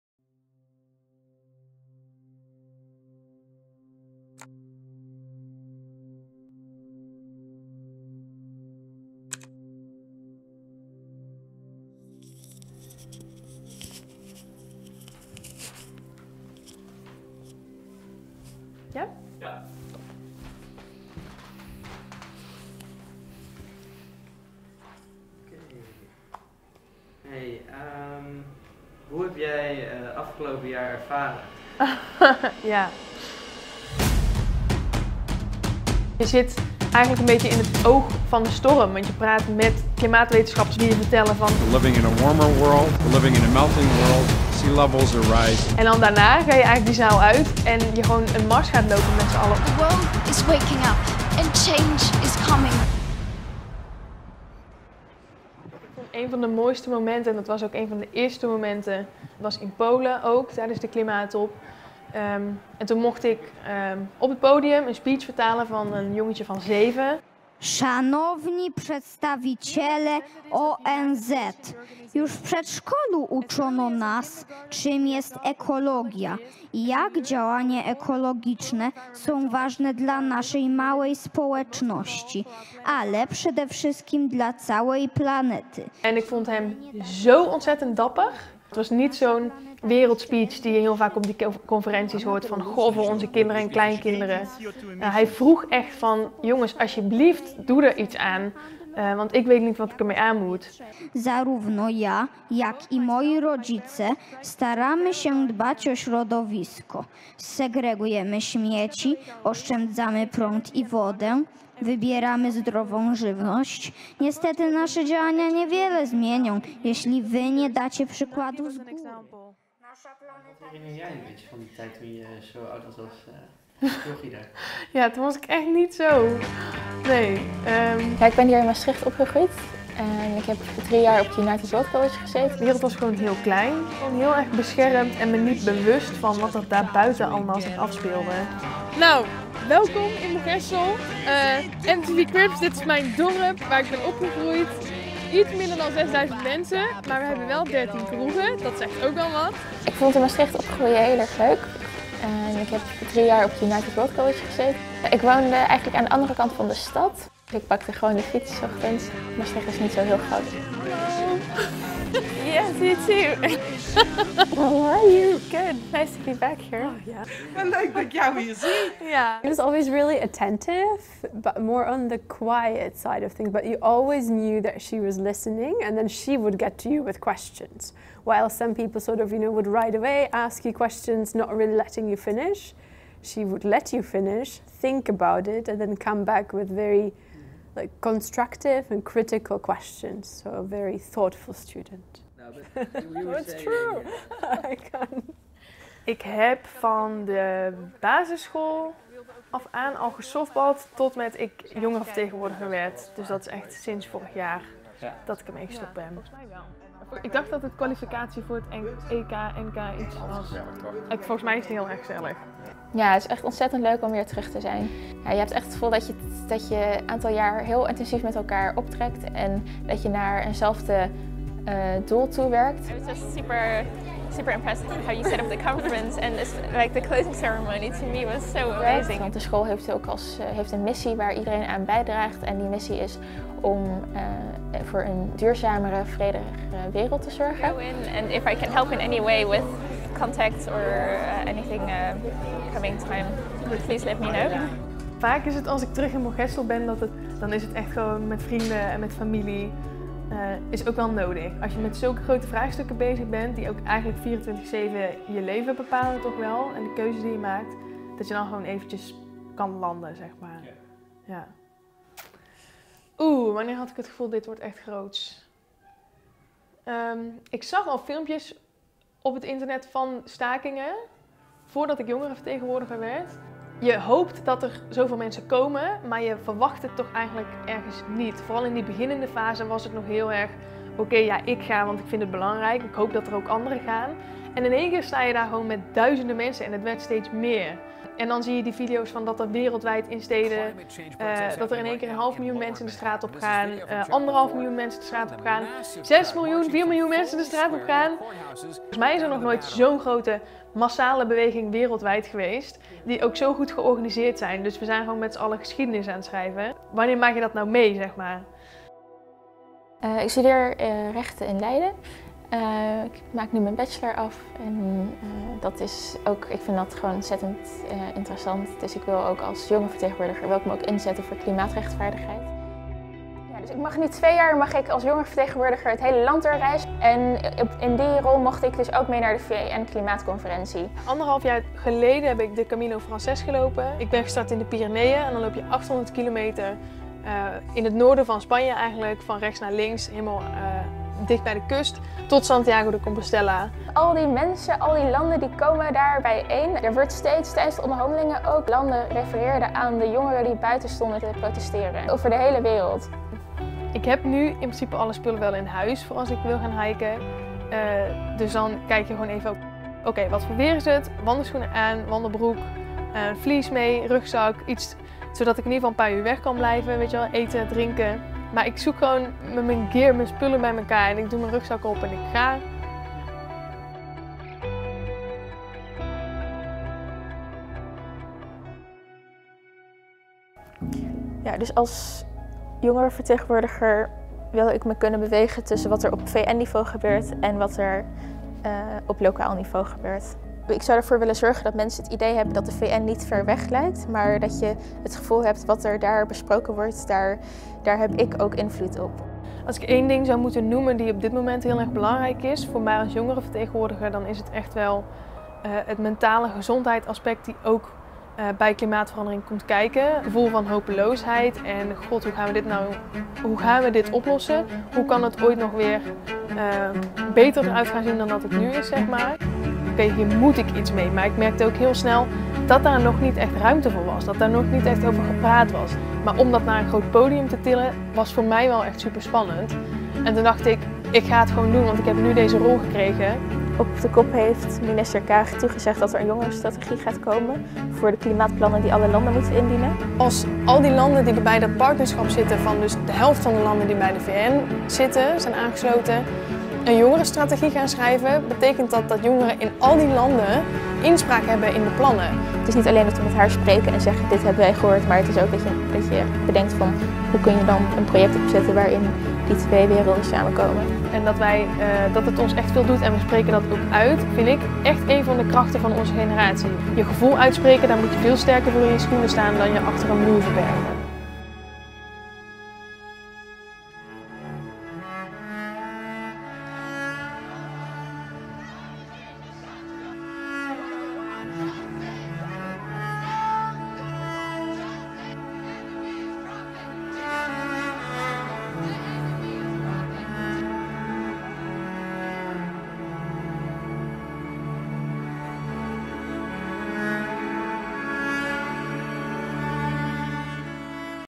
Mini doc werkgroep Jong en Duurzaam
NJR's werkgroep Jong en Duurzaam in de spotlight! Deze documentaire is gemaakt door werkgroeplid Job Verberne

Meer lezen over de werkgroep Jong en Duurzaam?